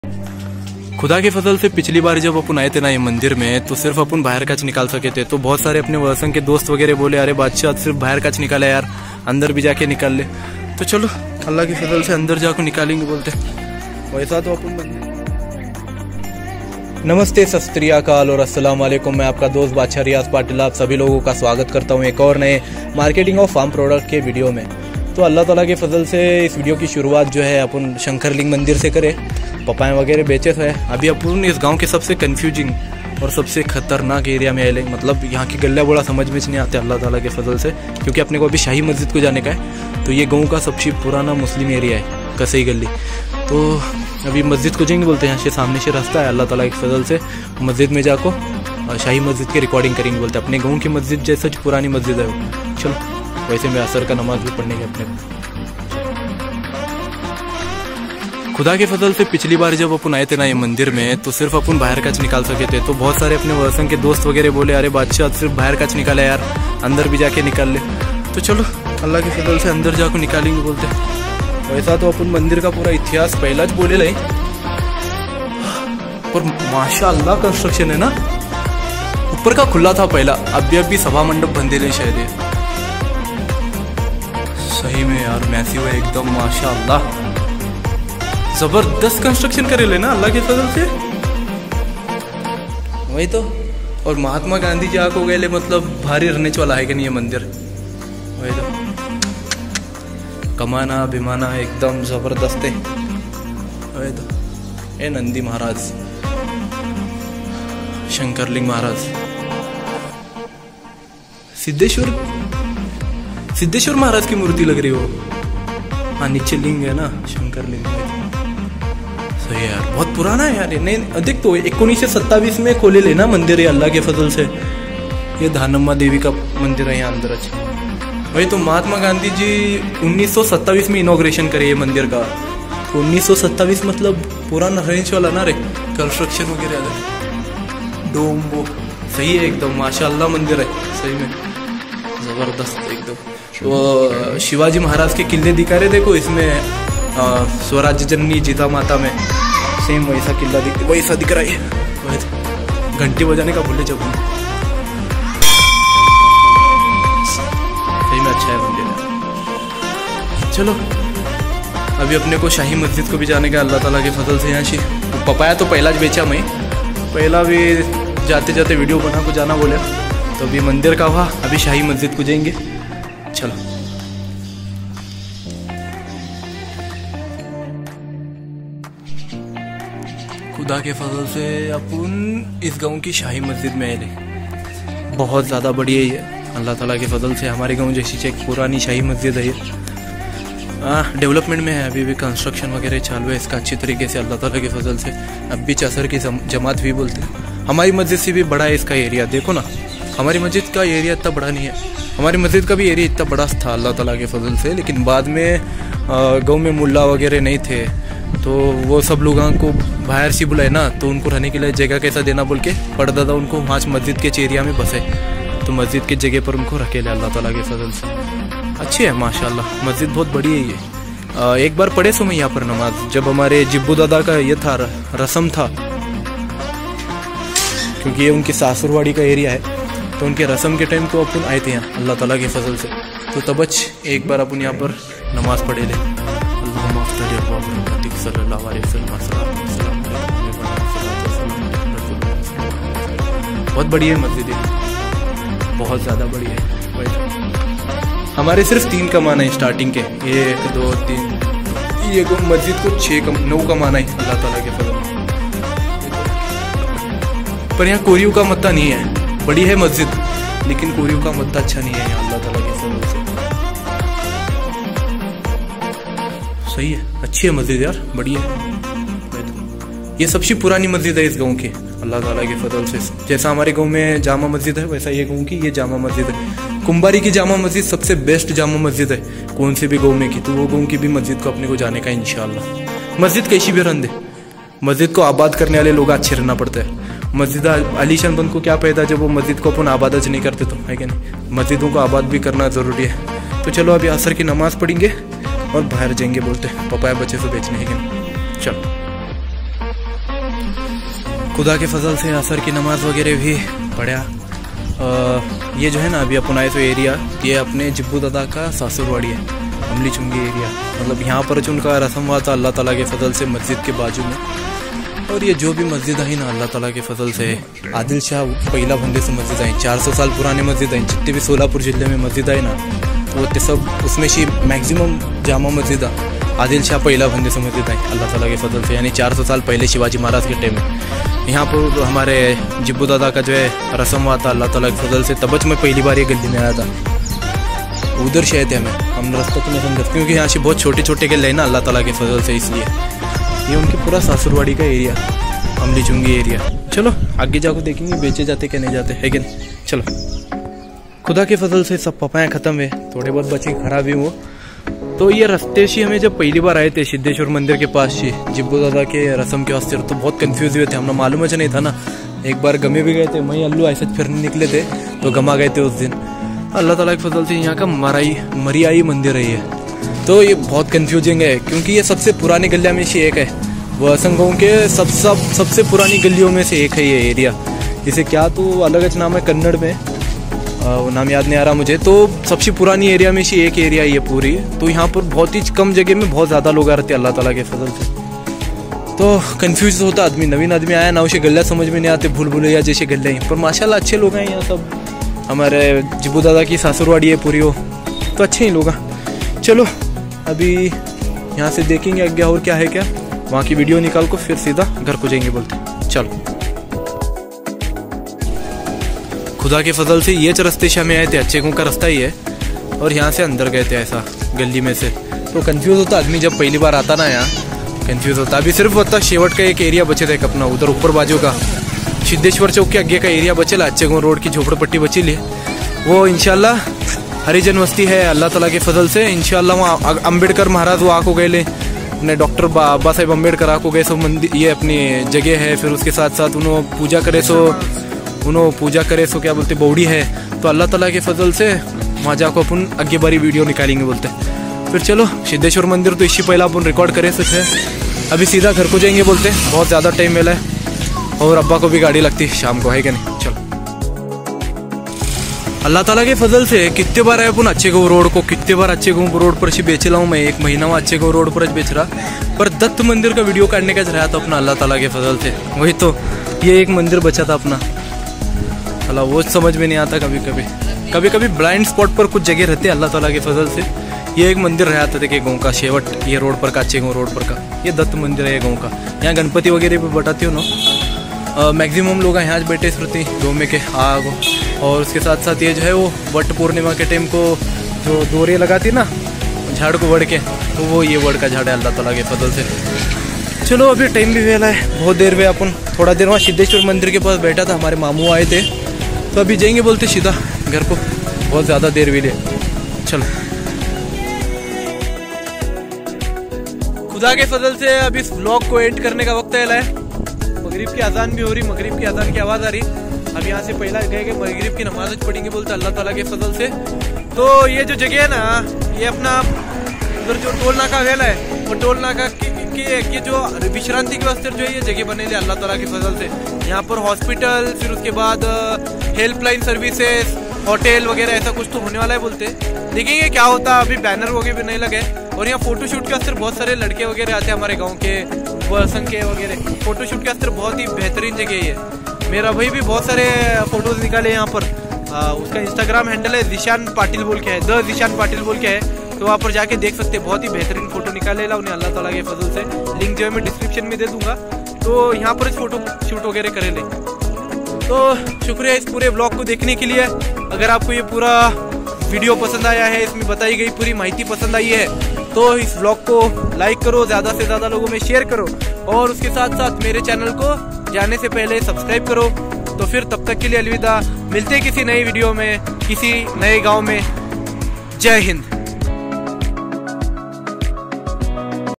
वळसंग में ये ना देखा तो क्या 🤔 देखा ?
Welcome to another Farm Product Marketing Vlog.

In today's vlog we are going to Valsang Village for marketing of Papaya.

I am going to Valsang Village after long time & it was a great experience.

Today's Special :-

श्री शंकरलिंग देवालय 🙏.
शाही जामा मस्जिद.
दत्त मंदिर.

This video is created By following all the Covid-19 rules.

Farm Product :- Papaya.

Marketing Date :-‌ 26 Jul. 2021.

Sell Of - 30 Papaya For 200 Rs.

Chapters :-

00:00 - आगे देखिये 👉👉👉👉
00:15 - सफर की शुरुवात.
00:56 - श्री शंकरलिंग देवालय 🙏.
02:58 - शाही जामा मस्जिद.
04:58 - जुबेर दादा 🧔 की सासुरवाडी 🏡.
06:47 - आचेगाव रोड 🛣️.
07:34 - दत्त मंदिर, वळसंग.
08:25 - सफर की समाप्ती.

Please Follow My Brother's Instagram :-

Created By :- Badsha Riyaz Patil.